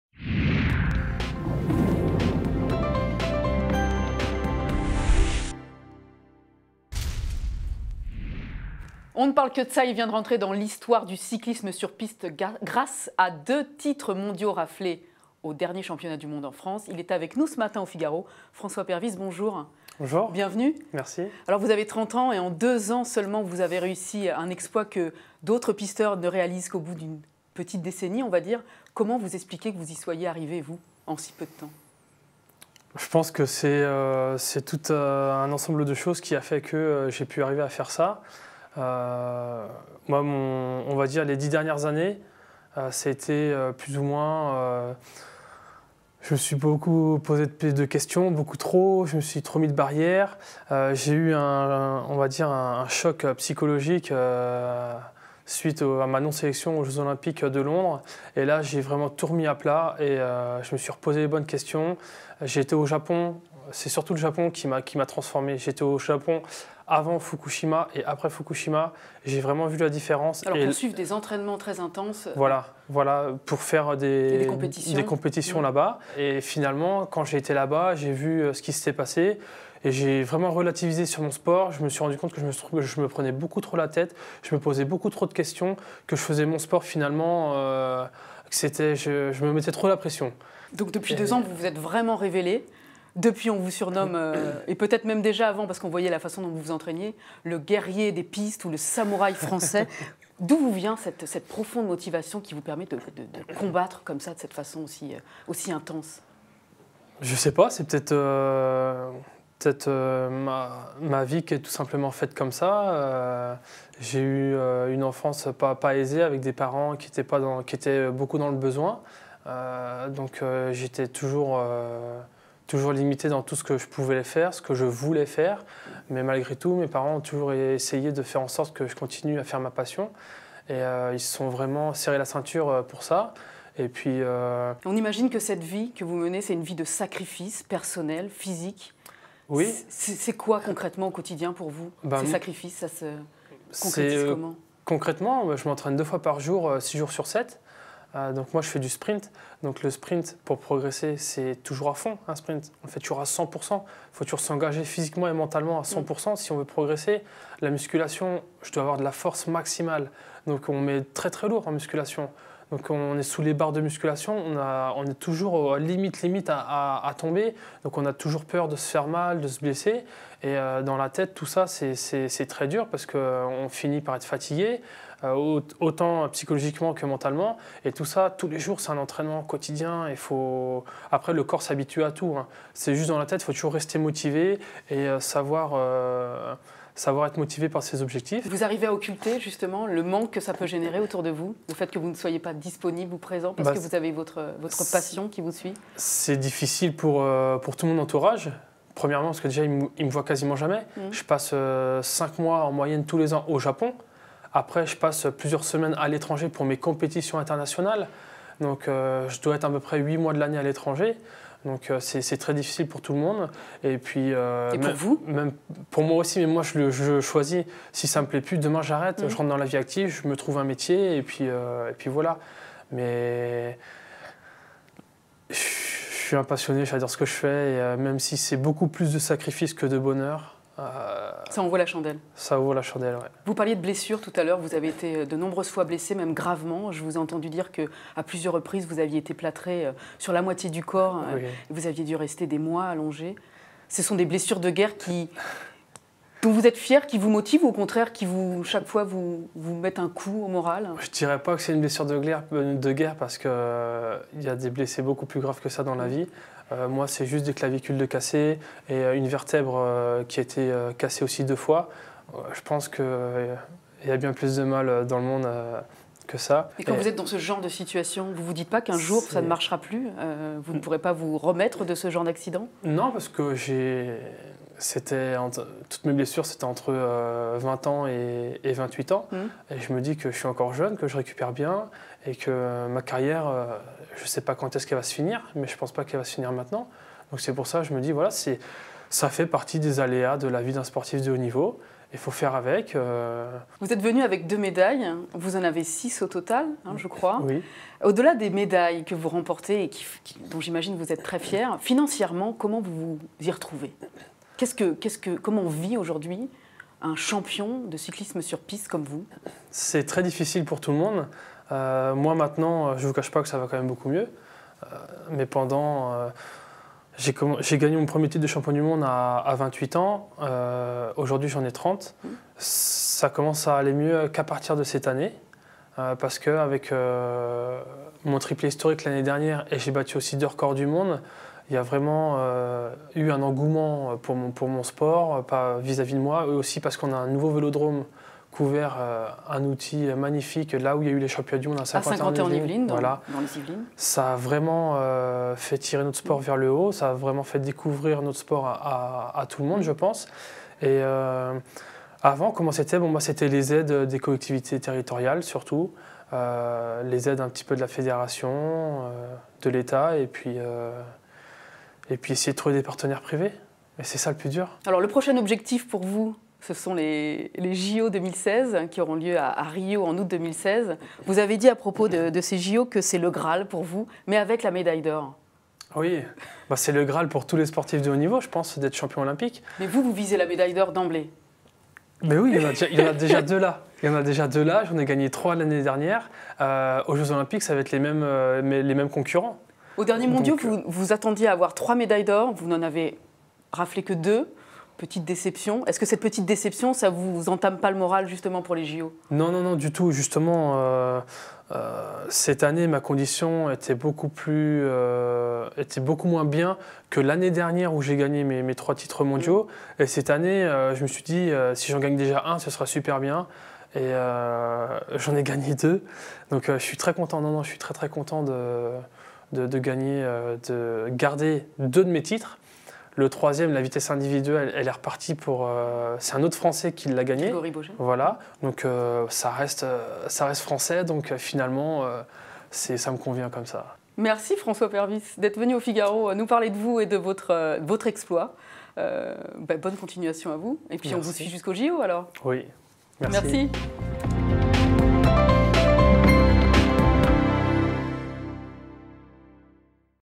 Cyclisme : «C'est plus de sacrifices que de bonheur» confie François Pervis
À 30 ans, il vient d'entrer dans l'histoire du cyclisme sur piste grâce à ces six titres de champion du monde dont deux raflés aux Mondiaux 2015. Pour le Figaro TV, il se livre sur ses exploits et les sacrifices qu'implique sa carrière.